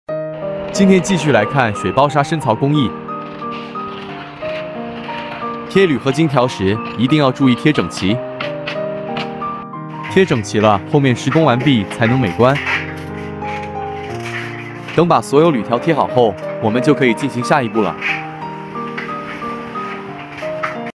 石漆水包砂
67.Today, we will continue to look at the water in sand groove production process. When pasting aluminum alloy strips, we must pay attention to their neatness. Only after the construction is completed can they be aesthetically pleasing. After pasting all the aluminum strips, we can proceed to the next step.